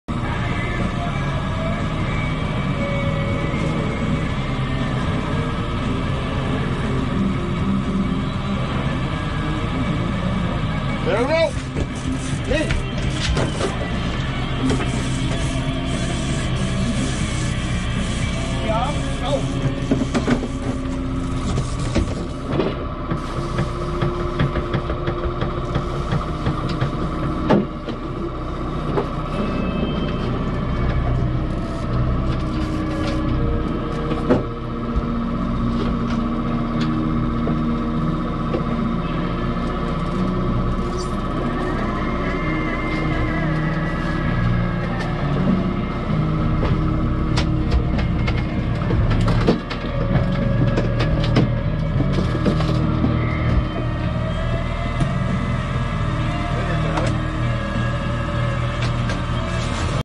Willamette River Coho September 16th 2019
Takedown to net hatchery Coho T Bruce Custom Rod ,T Bruce Custom Flasher and a T Bruce rigged Brads mini cutplug. It can be done!